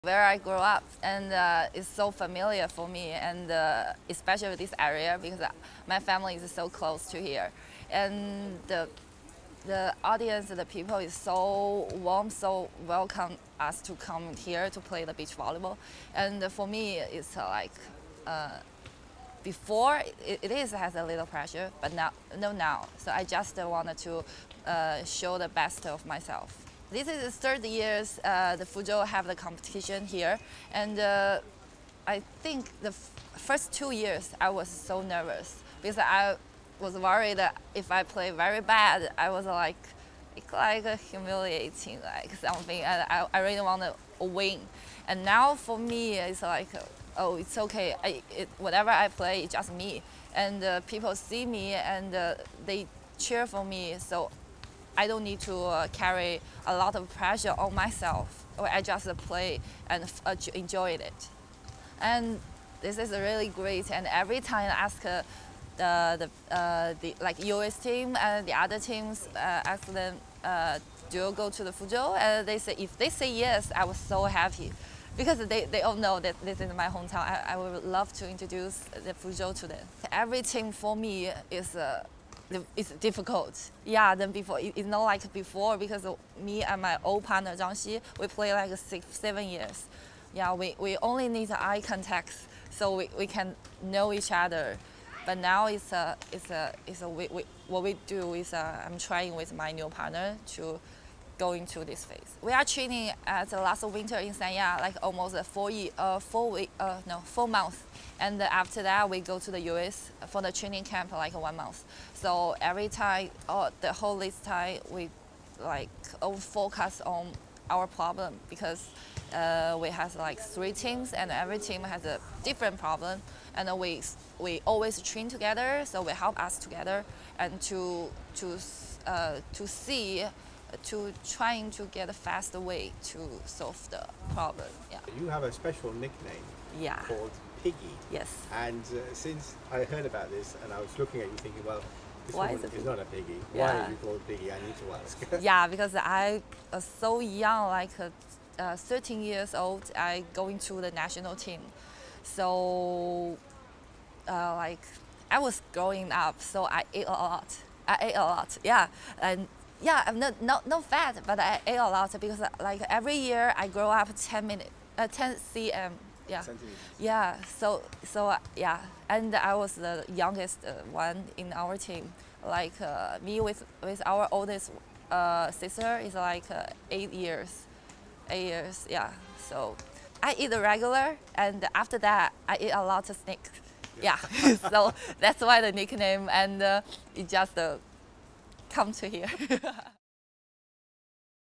BTS: Beach volleyball world champion Chen Xue on playing in front of her home fans
Behind the Scenes: 2013 FIVB Beach Volleyball World Champion Chen Xue talks to the Behind the Scenes crew on playing in front of her home crowd in Fuzhou as well as her plans for the upcoming season